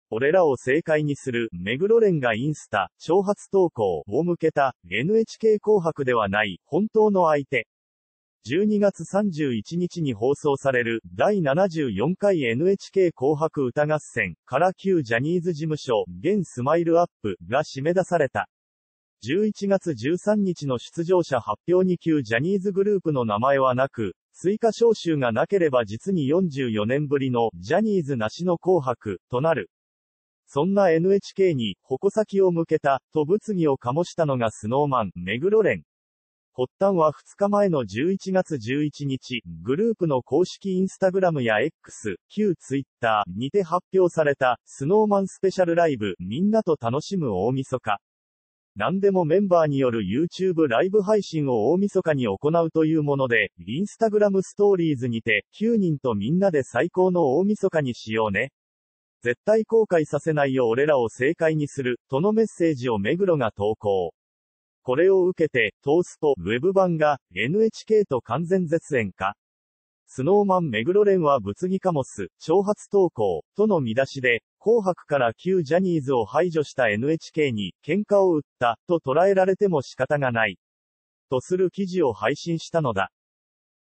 「俺らを正解にする」目黒蓮がインスタ“挑発投稿”を向けた、NHK紅白ではない「本当の相手」
12月31日に放送される『第74回NHK紅白歌合戦』から旧ジャニーズ事務所（現SMILE-UP.）が締め出された。11月13日の出場者発表に旧ジャニーズグループの名前はなく、追加招集がなければ実に44年ぶりの“ジャニーズなしの紅白”となる。
そんなNHKに“矛先を向けた”と物議を醸したのがSnow Man・目黒蓮。発端は2日前の11月11日、グループの公式インスタグラムやX（旧ツイッター）にて発表された『Snow Man Special Live～みんなと楽しむ大晦日!～』。 　何でもメンバーによるYouTubeライブ配信を大晦日に行うというもので、インスタグラム・ストーリーズにて《9人とみんなで最高の大晦日にしようね!　絶対後悔させないよ　俺らを正解にする》とのメッセージを目黒が投稿。 　これを受けて『東スポ』Web版が、【NHKと完全絶縁か…Snow Man 目黒蓮は物議醸す“挑発投稿”】との見出しで、紅白から旧ジャニーズを排除したNHKに【“ケンカを売った”ととらえられても仕方がない】とする記事を配信したのだ。

IMPORTANT!